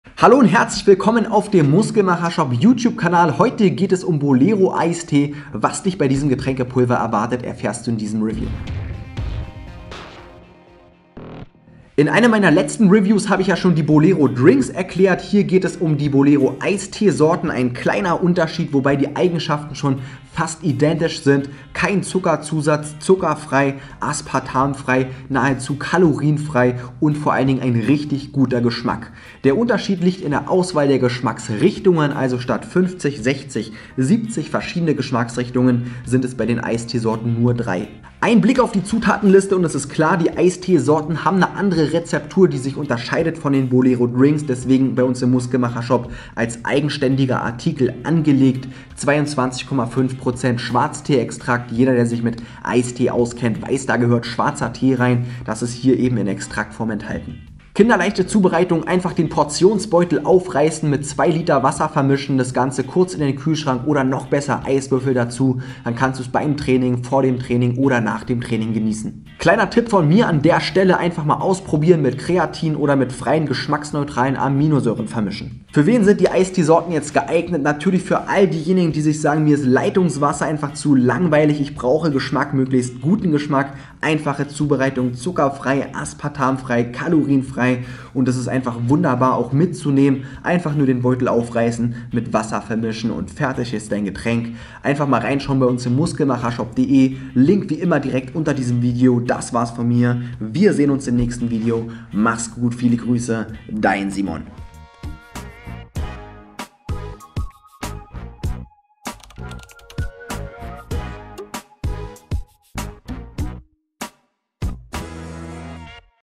Bolero Ice Tea Drinks 🍹 im Review | Muskelmacher Shop
Die Bolero Ice Tea Sorten sind ein Bestseller im Muskelmacher Shop. Leckerer Geschmack, keine Kalorien und kein Zuckerzusatz machen die Getränkepulver so beliebt. Im Unterschied zu den Bolero Drinks gibt es nur drei Geschmacksrichtungen. Simon erklärt was dich bei den leckeren Eistee Sorten erwartet!